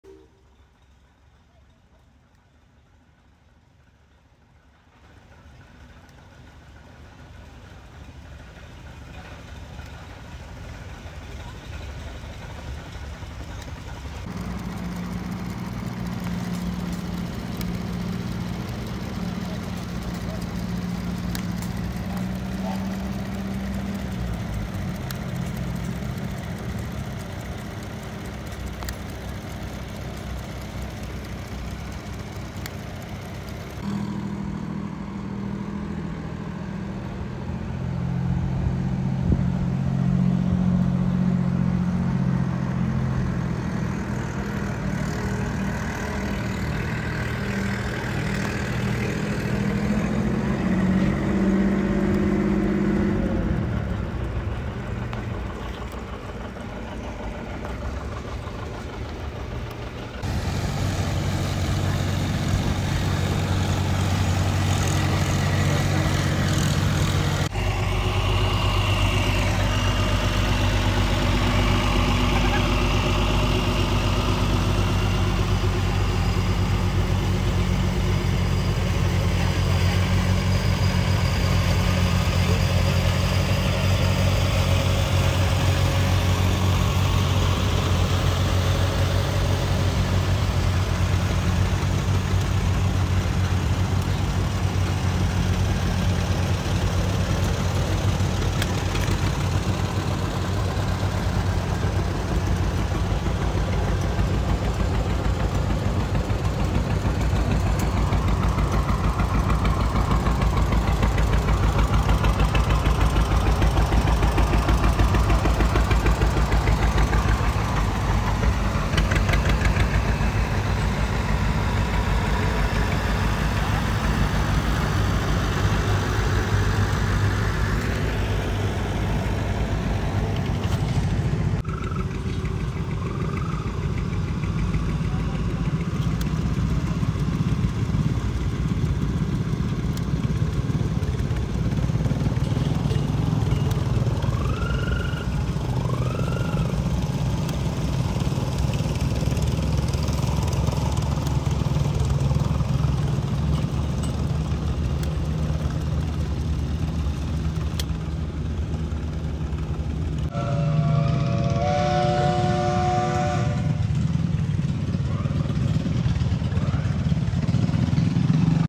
June Tractor Show 2023
Hello Everyone! I hope you enjoy this video. The video was shot at Rough and Tumble during what used to be the IH Days. For 2024 the show will now be Rough and Tumble's Ag Days. I hope you enjoy this video.

A little information on the camera that was used

This Video was something new that I tried out. I used a Bolex H8 8mm video camera. This was the first time using the camera! For a extra twist I recorded sound on my phone and then edited the video and sound together so that way there is sound of what is being filmed. Some of the sound may not be perfectly lined up, as this was my first time doing something like this.

Thank you @FilmPhotographyProject  for having the film available for purchase and also for developing and scanning the film so I not only have it on my computer to do something like this, but to also play it on my movie projector!